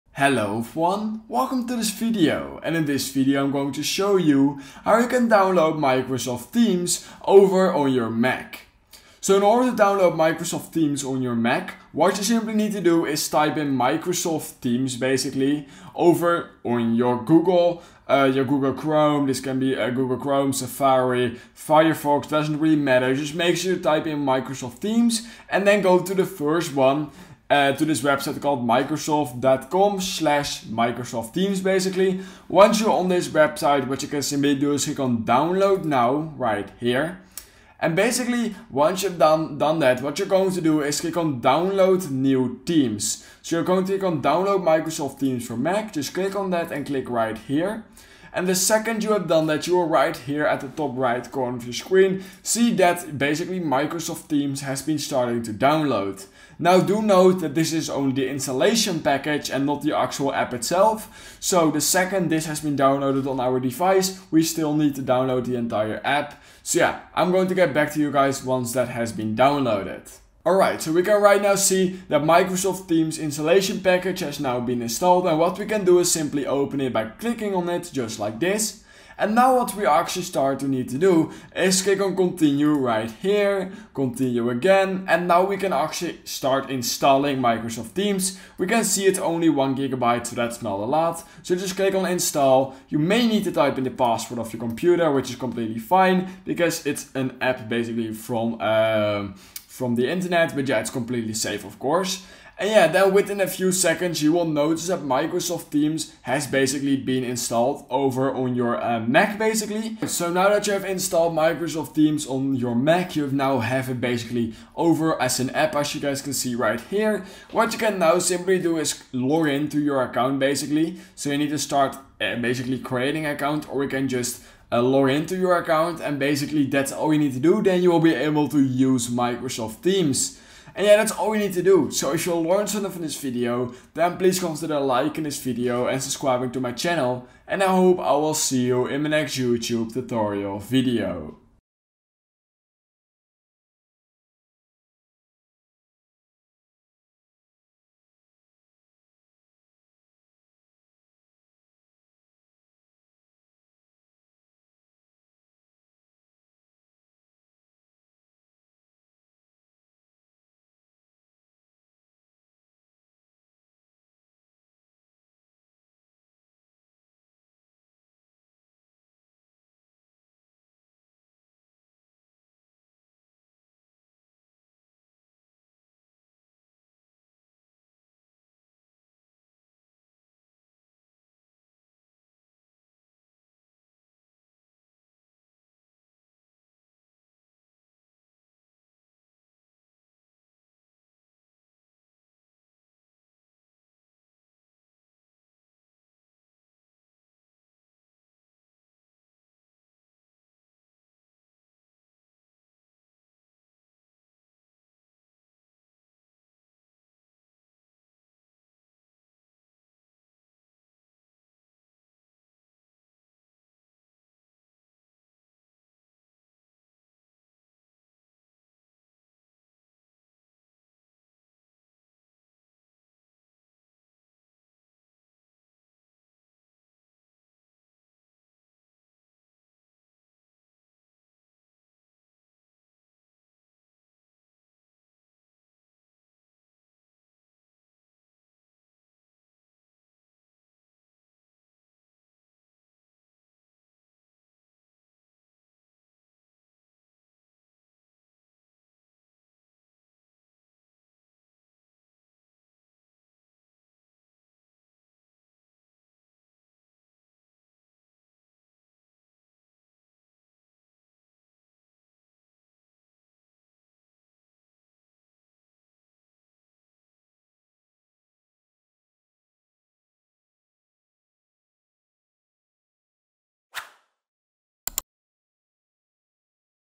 How To Download Microsoft Teams On Mac
How To Download Microsoft Teams On Mac - Full Tutorial

👉 In this video we are going to show you how you can download Microsoft teams on your Mac.